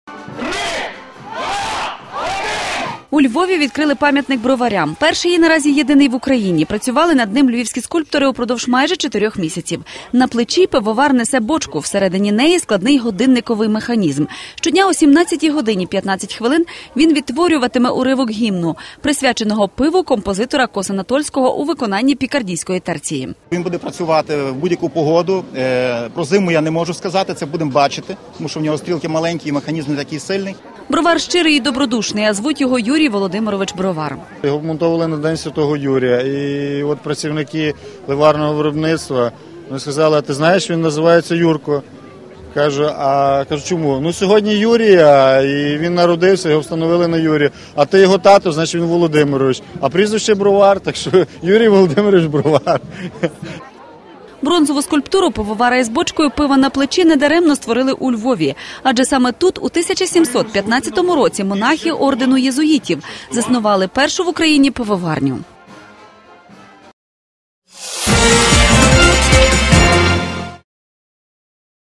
У Львові відкрили пам'ятник броварям
Цей пивовар - щирий і веселий. А чого ж йому ...